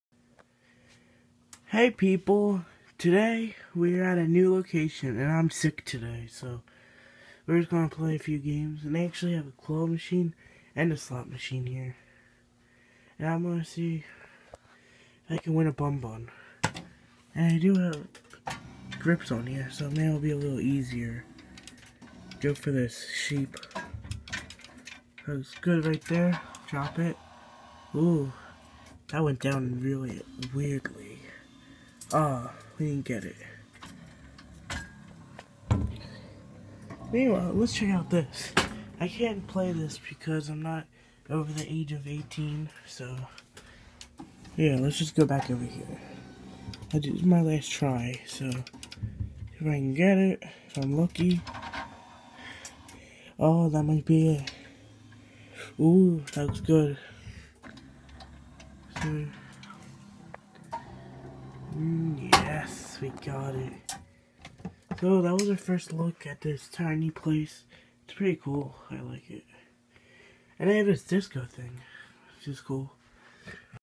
Zakk’s Claw Adventures Episode- 4- the groovy new location!
Today I find a new place that is lit by a disco ball and we win more product, for once not a play till you win claw!